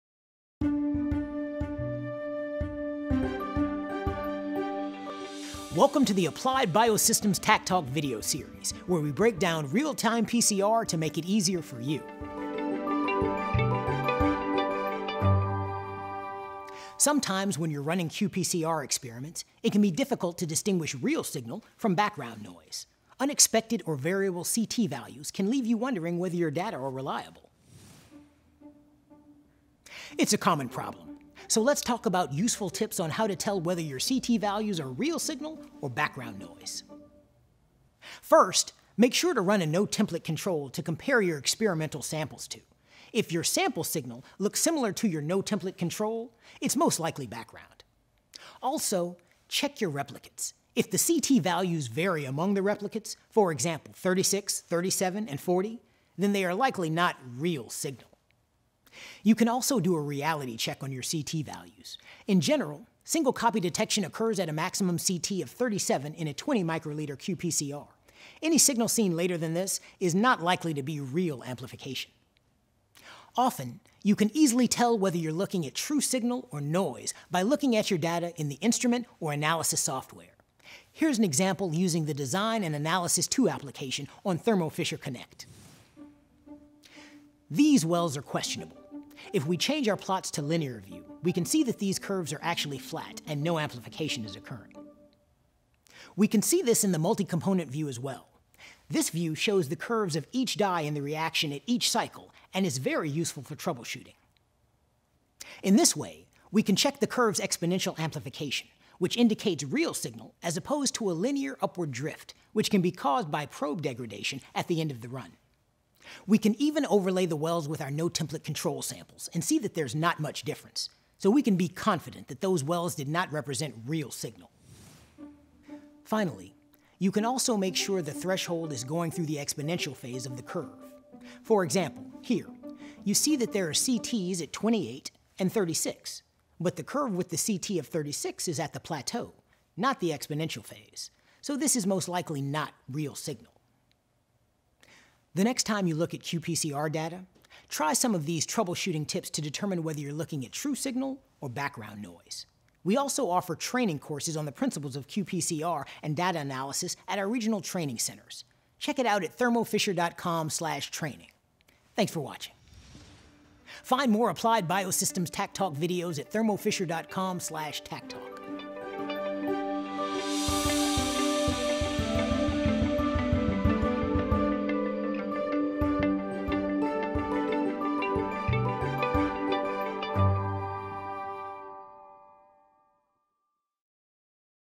How to tell if Ct values are real signal or background noise--Taq Talk Episode 14
It can be difficult to distinguish real signal from background noise. Let's review some troubleshooting tips to determine whether your Ct values true signal or background noise.